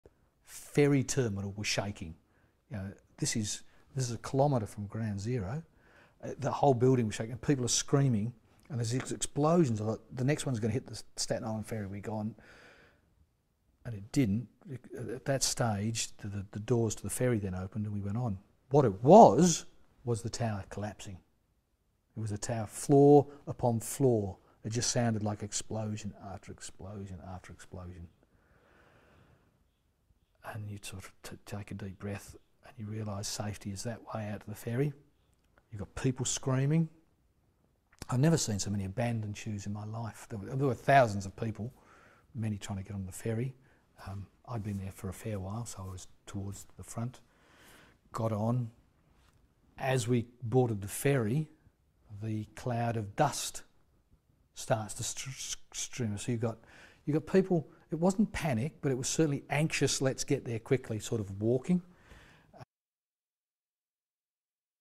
September 11th Ground Zero Explosions Witness Hans Kunnen
A Sydney man was one of the survivors of September 11. Hans Kunnen was at an economic conference at a hotel between the towers. Ten Years on he's told of how, in the horror of that day, he found an unlikely friend.